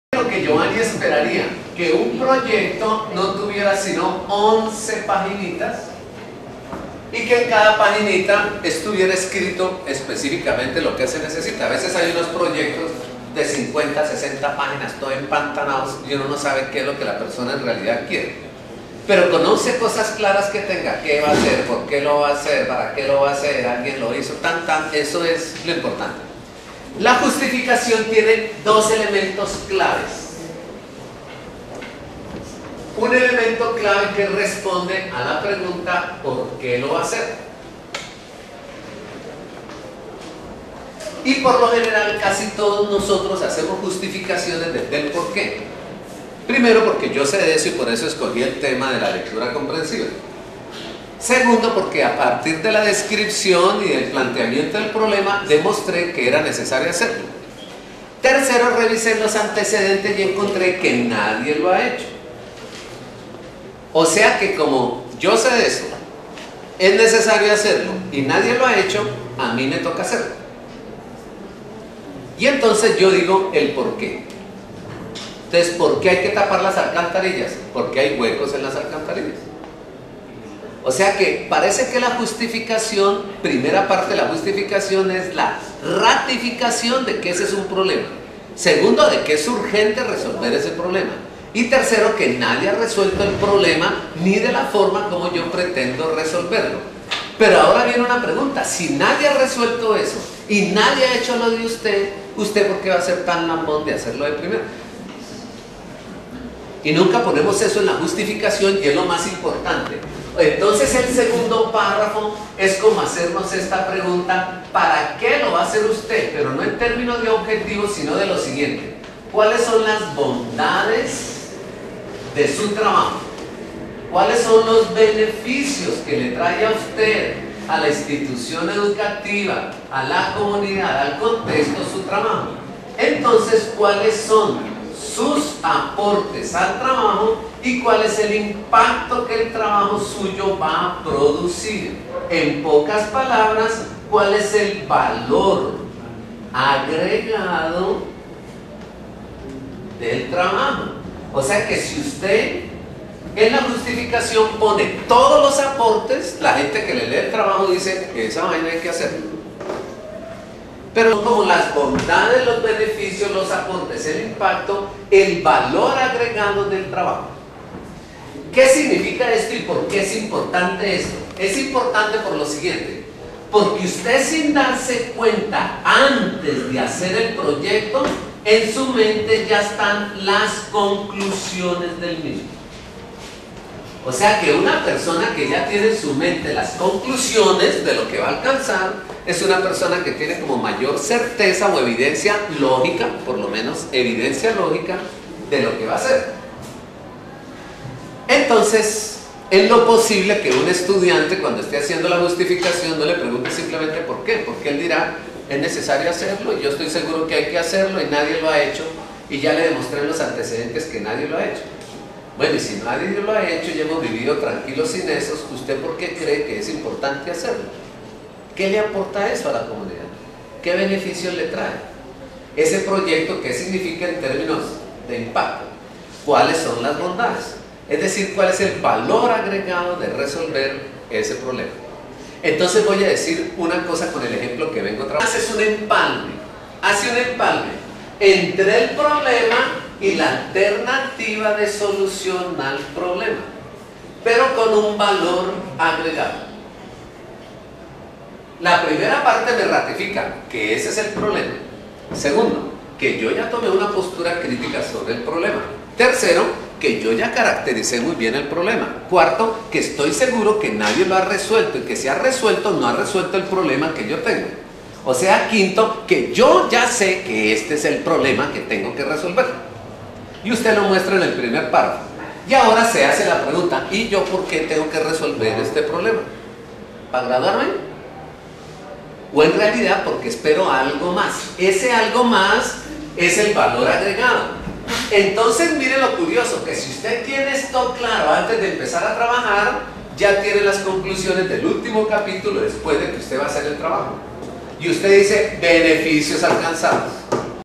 Estructuración de Proyectos de Investigación - Justificacion
El Profesor Giovanni Iafrancesco habla sobre la justificación en la estructuración de proyectos de investigación